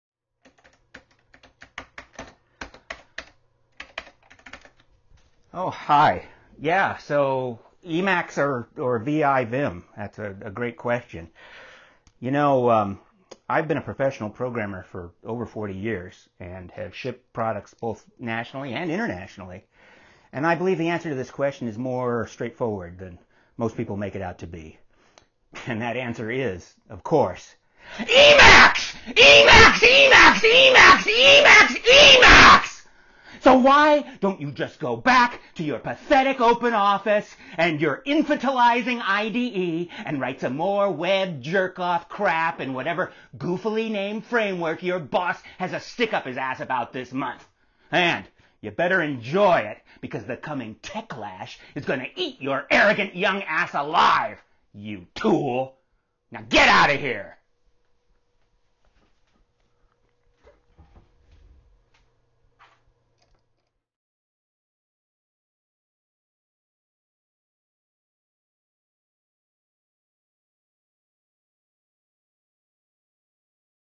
Emacs or VI: The Definitive Answer.
Which is better: Emacs or VI / VIM? After 40 years as a professional software developer working with both products, I'm happy to finally be able to present this reasoned and definitive answer (as well as some advice for younger programmers starting their careers in our industry).